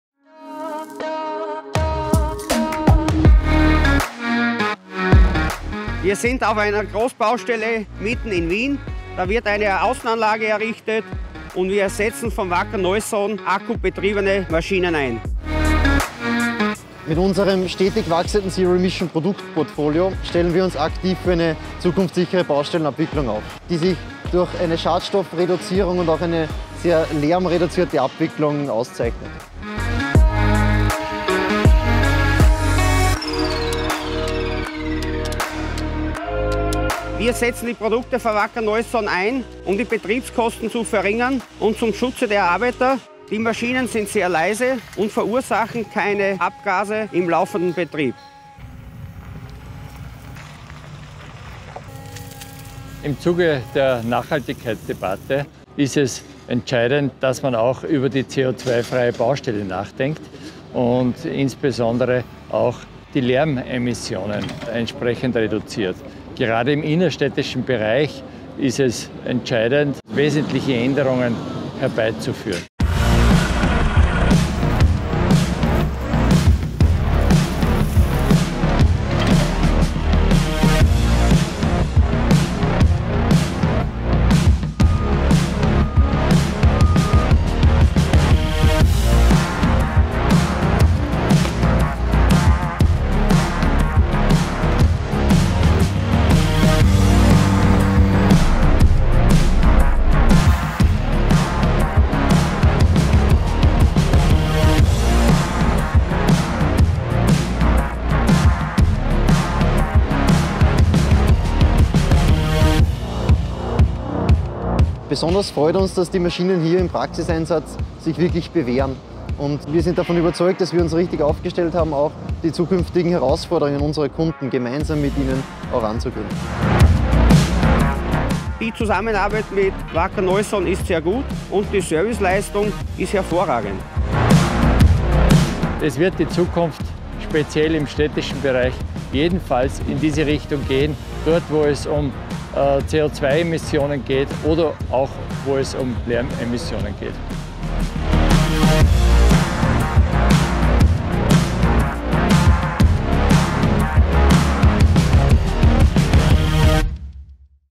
The construction site of the future – Emission-free and low-noise in the middle of Vienna
Within the framework of a private construction project in which a building complex of high-rise residential buildings, an office building and a hotel are developed, the outdoor facilities are designed with the help of the zero emission range by Wacker Neuson.

From internal vibrator through the vibratory rammer to the dumper and mini-excavator, the battery-powered machines have proven in a central location that they are ideally suited for the daily practical application - and furthermore, for flexibility, as well as operator and environmental protection.

Wacker Neuson is proud to be a global brand. Not all products, configurations or services shown here may be available in your market.